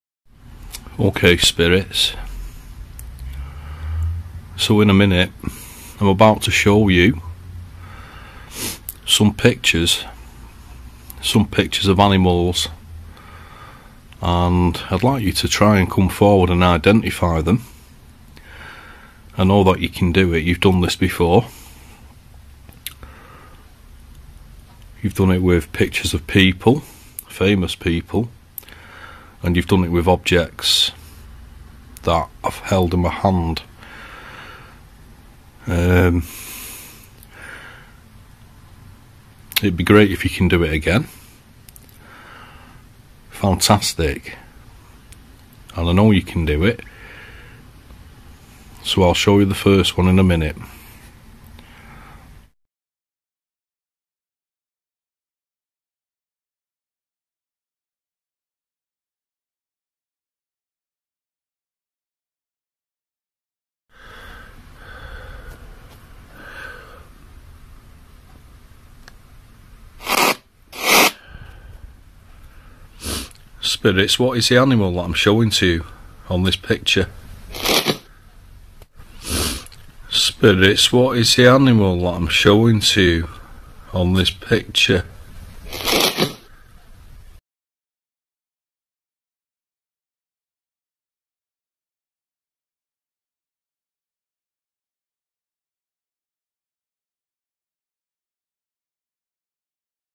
GHOST'S LOOK AT PICTURE OF A HEDGEHOG AND IDENTIFY IT.
Another example of spirits being able to use a spirit boxes frequencies, manipulate them and answer questions.
In this demonstration I attempted to get spirits to identify an animal on a picture,  it isn't easy, but they actually did it very fast,  but subsequently after identifying the hedgehog they didn't identify any other pictures I shown them,  so in this example I show them identity the hedgehog.